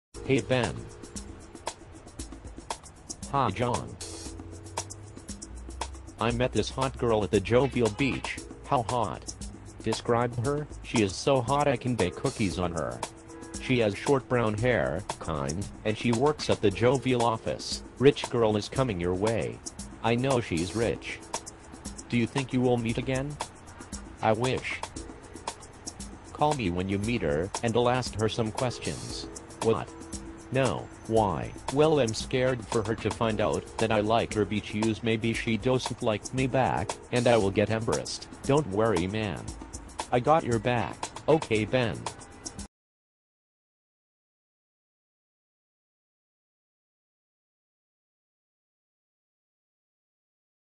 John and Natasha 3 - Who John Likes
Now John is in love with natasha! They are soulmates!!